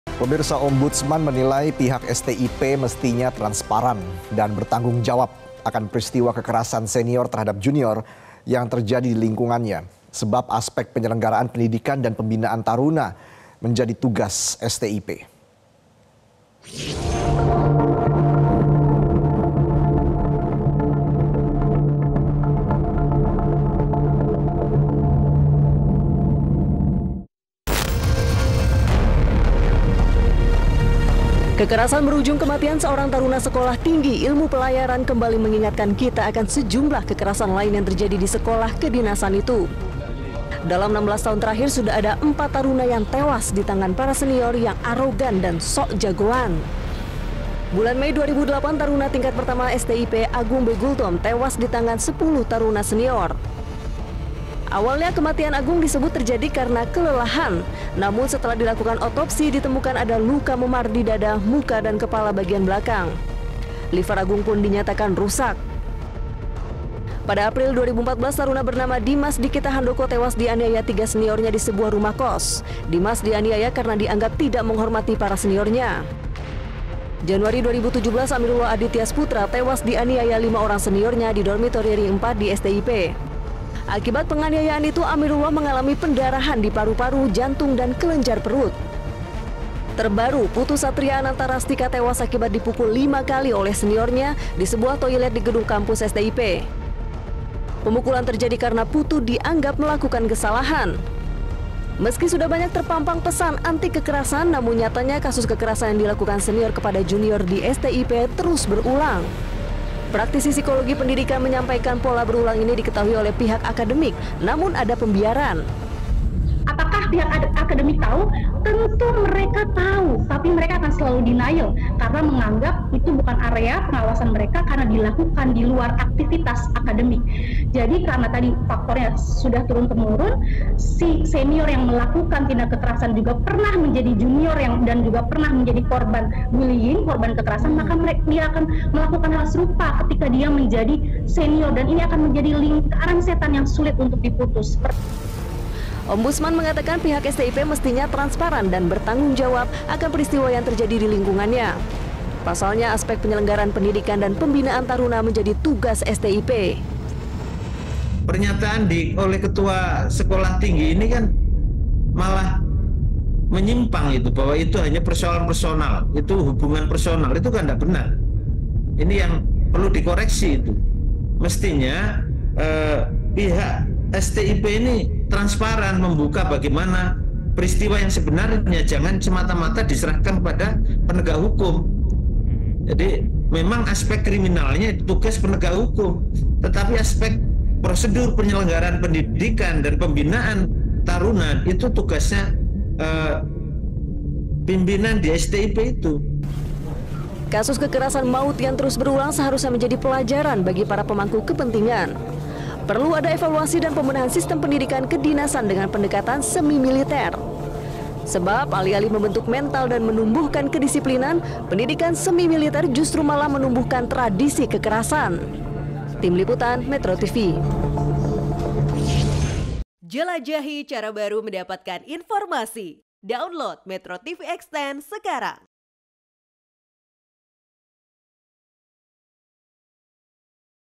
Kasus P3ng4niay44n di STIP Terus Berulang, Ombudsman RI Minta STIP Tanggung JAwab
MetroTV, K45u5 p3ng4niay44n senior terhadap junior hingga t3w4s di Sekolah Tinggi Ilmu Pelayaran (STIP) terus berulang. Baru-baru ini, siswa STIP, Putu Satria, di4ni4ya seniornya hingga tewas.

Kasus serupa pernah muncul pada 2008, 2014, 2015, dan 2017. Ketua Ombudsman RI, Mokhammad Najih, mengatakan hal ini menjadi bukti permasalahan penegakan hukum tak berjalan.